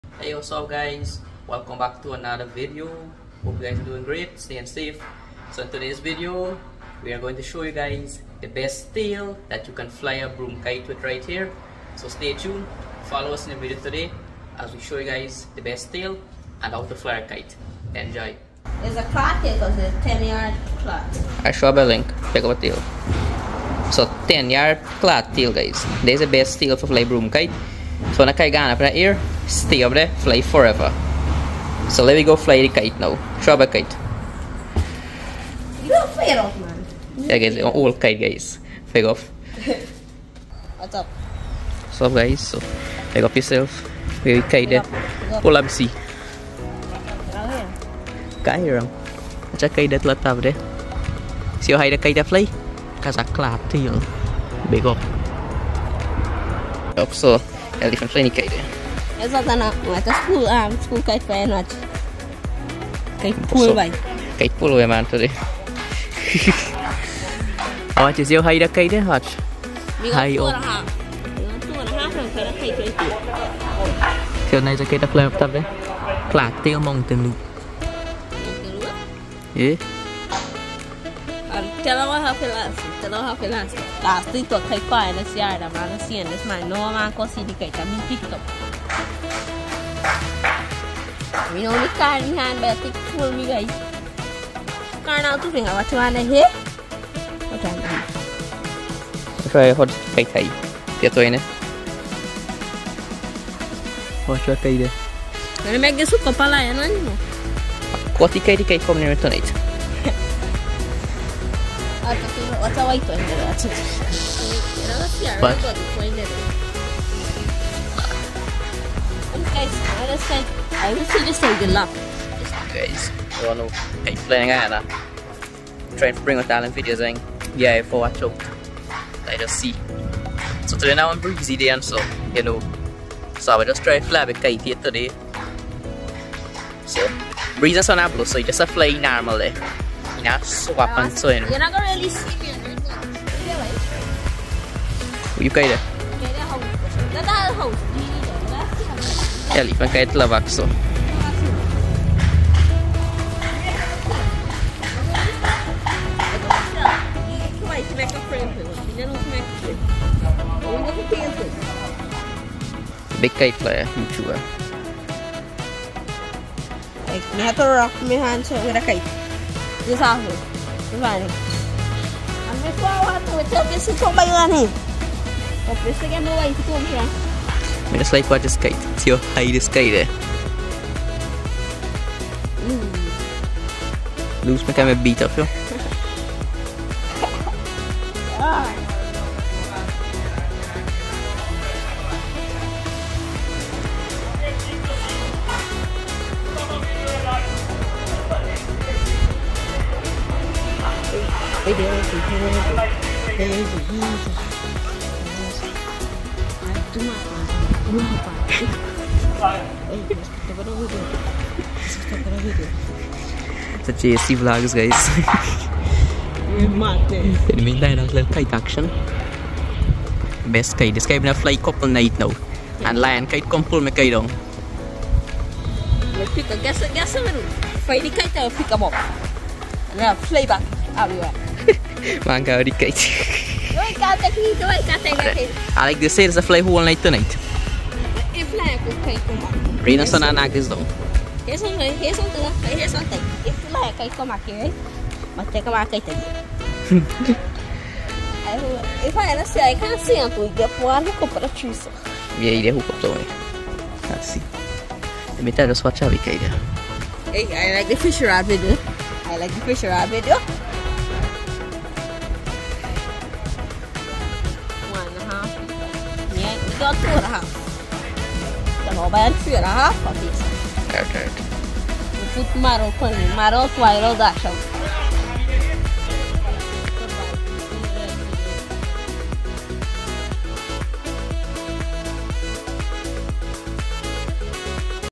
Best Tail For Flying A Guyanese Broom Kite | Guyana Kite Flying (2024)
Kite Flying Guyana